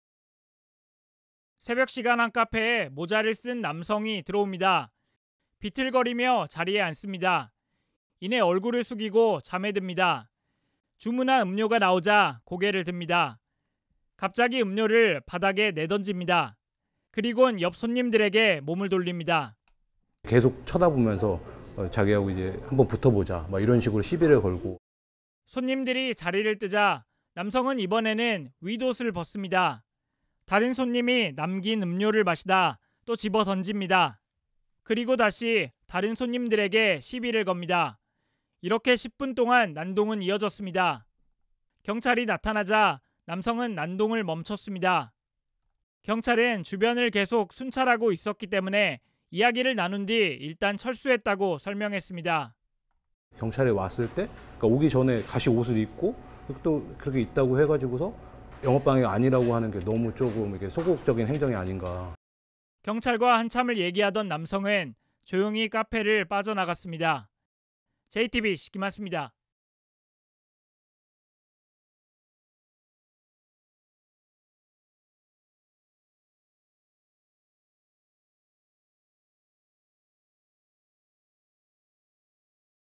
[자막뉴스] 옷 벗고 '나랑 붙자' 하더니…경찰 오자 보인 행동 / JTBC News
새벽 시간, 카페에 들어온 한 손님이 비틀대며 난동을 부리는 모습이 또 포착됐습니다. 자신의 음료를 집어던지고 다른 손님들에게 시비를 걸다, 경찰이 출동한 끝에 멈췄습니다.